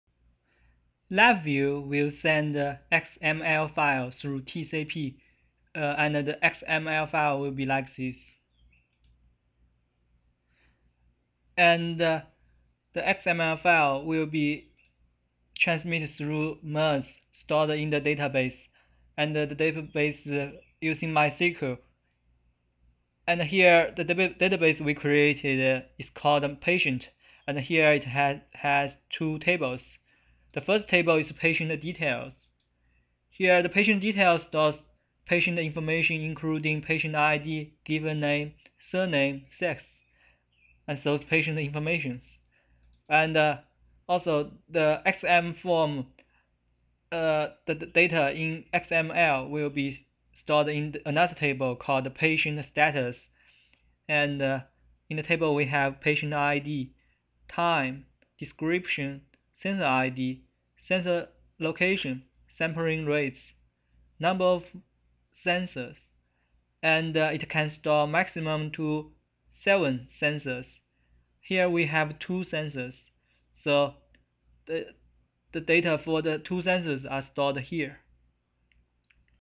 Back-end Database Design
The initial Prototype Design of Dynamic EHR Sensor Interface was built, using a PDA as Dynamic EHR Sensor Interface. In the back-end, data are stored in a MySQL database. We implement the back-end EHR system as MIRTH (an open source cross-platform HL7 interface engine that enables bi-directional sending of HL7 messages between systems and applications over multiple transports) with an attached MySQL database rather than build a specific open-source EHR.